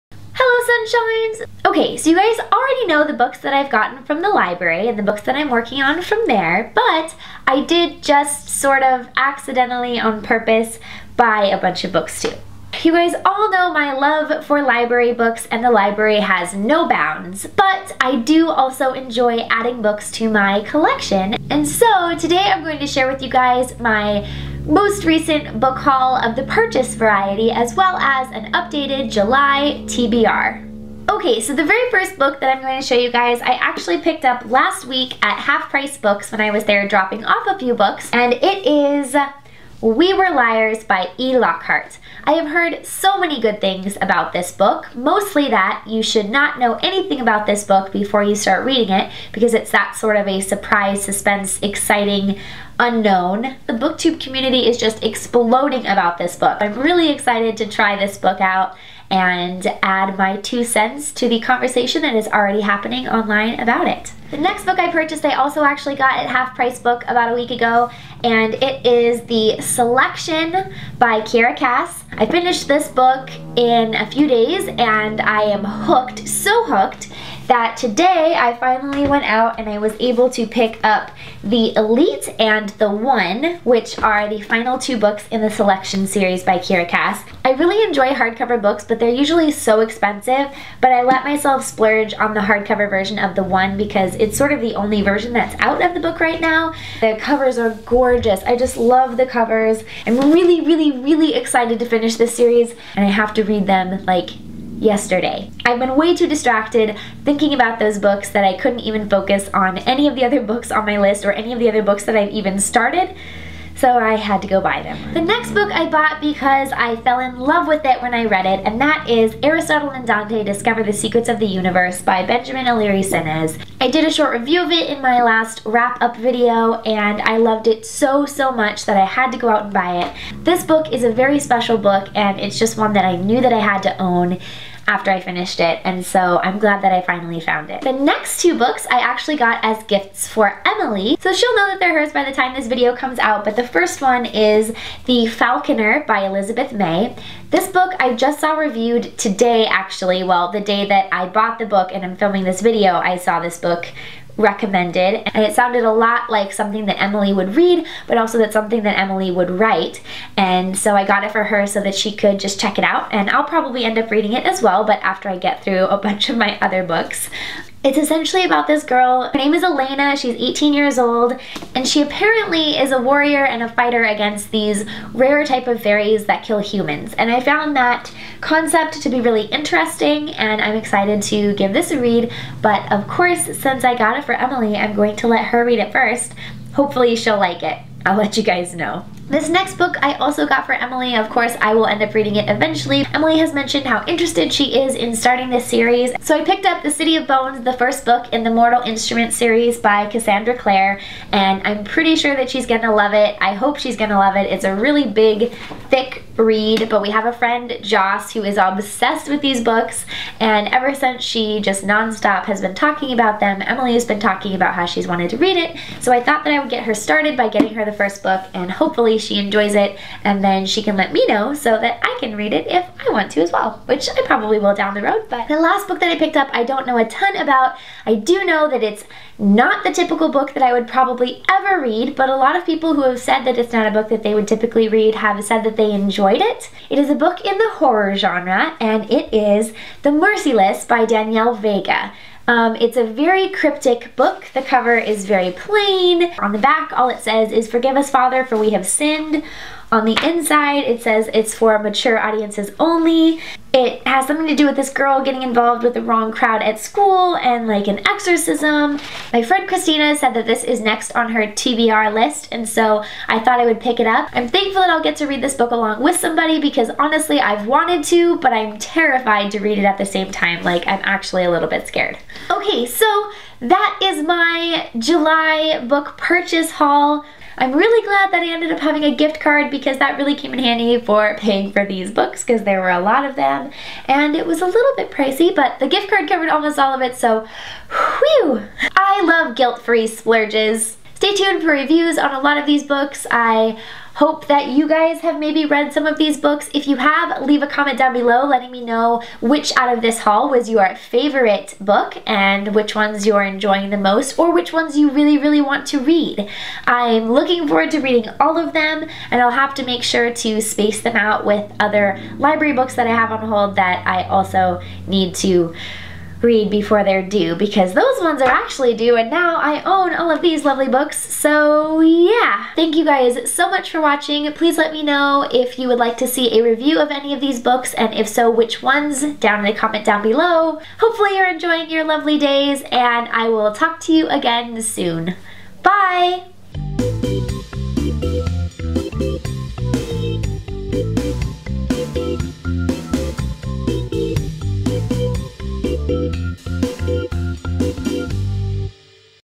July Book Haul & TBR (2014)
Though I'm new to the BookTube community I figured I might as well dive in head-first!  This video features the purchased books I've hauled and is an updated version of my July TBR!

Have you read any of these books?  Do you want to read any of these books?  (No spoilers please!)

Like what you see and want to support me making more videos and get some fun perks at the same time?:

I N S T A G R A M :
malaray